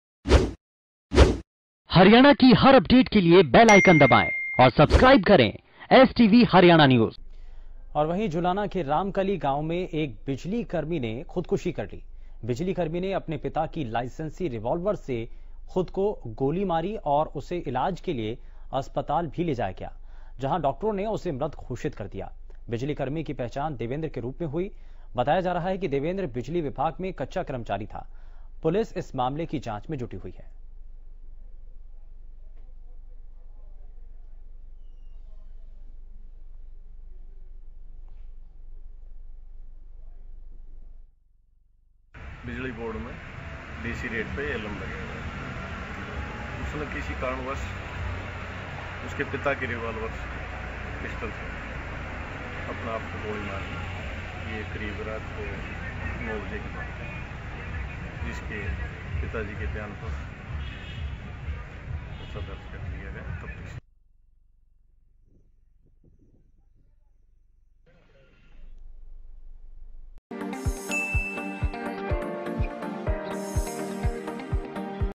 जुलाना में बिजलीकर्मी ने की खुदकुशी, रिवॉल्वर से खुद को मारी गोली || STV Haryana News
जुलाना के रामकली गांव में एक बिजलीकर्मी ने खुदकुशी कर ली. बिजलीकर्मी ने अपने पिता की लाइसेंसी रिवॉलवर से खुद को गोली मारी. उसे इलाज के लिए अस्पताल भी ले जाया गया. जहां डॉक्टरों ने उसे मृत घोषित कर दिया.